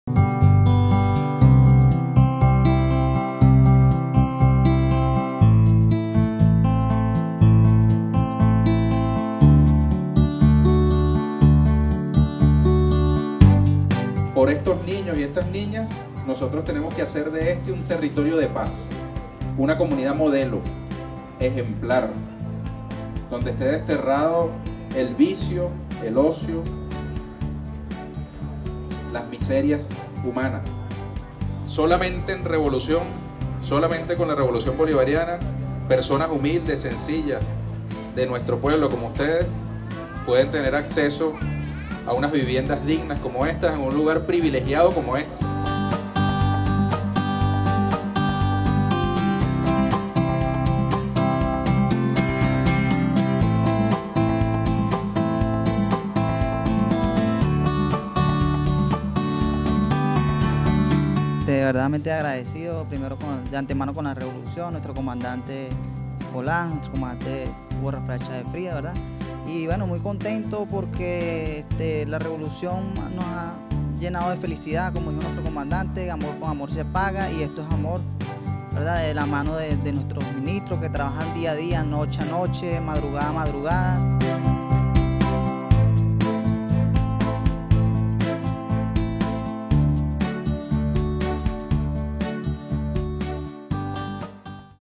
Entregados 56 nuevos aptos en el conjunto Oscar López Rivera
El ministro de Estado para la Transformación Revolucionaria de la Gran Caracas, Ernesto Villegas, y el director de la Comisión Presidencial para Refugios Dignos, Francisco Vilela, entregaron este jueves 56 apartamentos del conjunto habitacional, Oscar López Rivera, ubicado en la avenida Bolívar de Caracas.